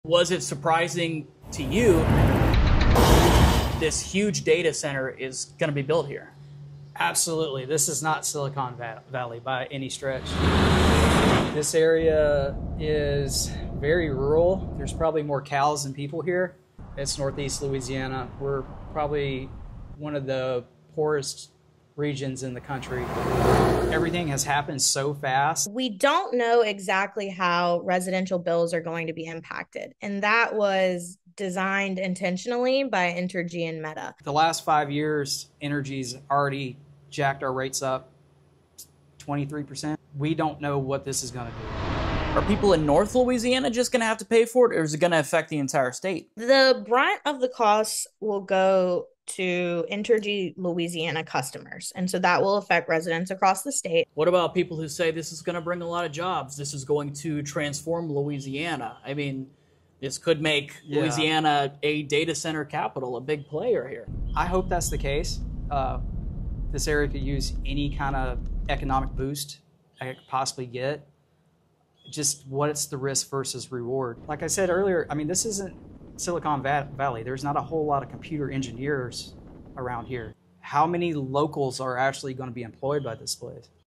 Louisiana’s new Roommate
Meta is living with Louisiana. The social media company is building its largest Ai data center in rural northeast Louisiana. Can the community handle this? And will it be a benefit, or a burden?